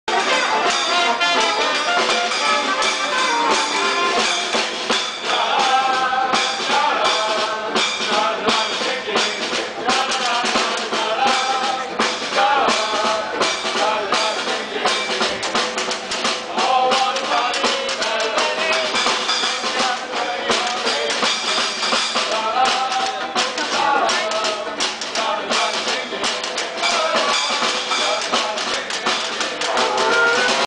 Slaughter House 7 at the Edinburgh Fringe Festival
This band was playing on the street at the edfringe. This is only a short clip of them busking.